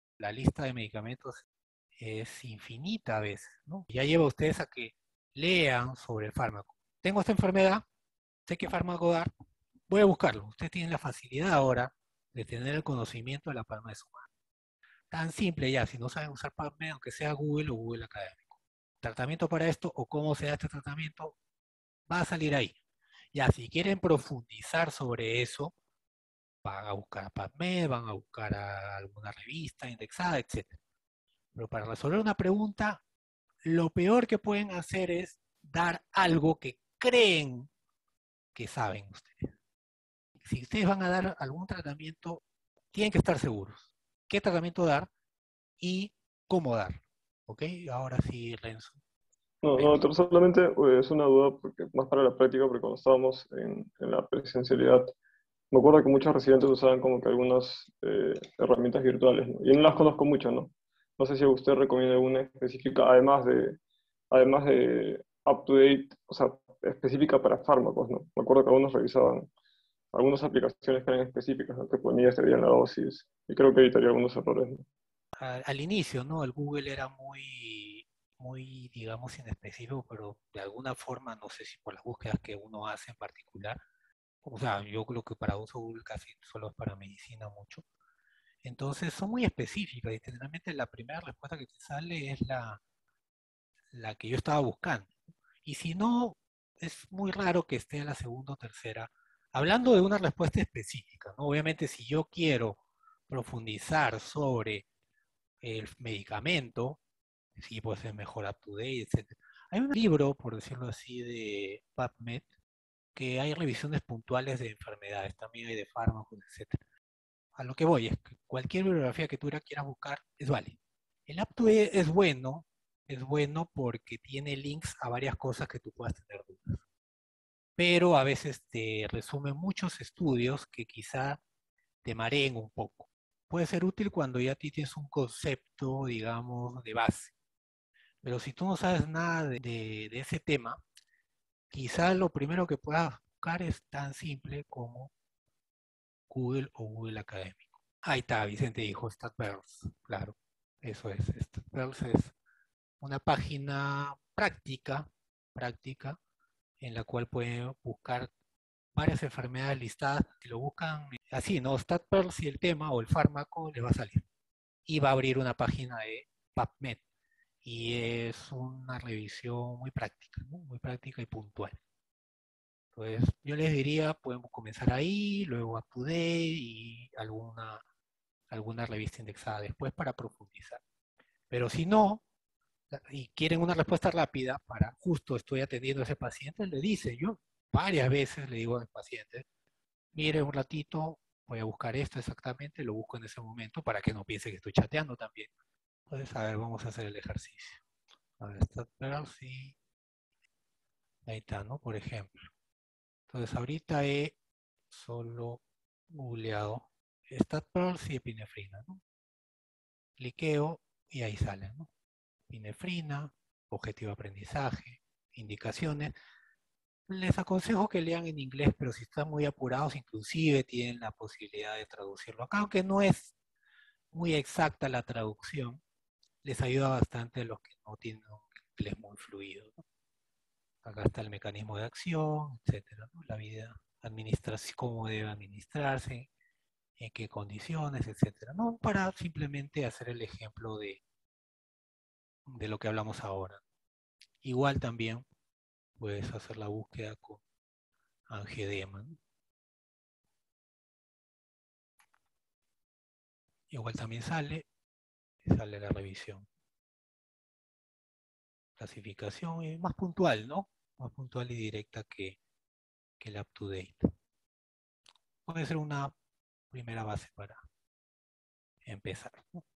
¿Dónde y cómo buscar bibliografía sobre fármacos? - PodCast 03 marzo 2022
Usar Google para estudiar medicina es malo?
“Lo peor que pueden hacer es indicar un fármaco que creen que conocen”.

Describimos la utilidad del StatPearls como bibliografía básica en medicina y hacemos un par de búsquedas en vivo.

0:00 ¿Cómo buscar sobre fármacos que desconocen?
1:49 StatPearls del NCBI
3:11 Ejercicio de búsqueda bibliográfica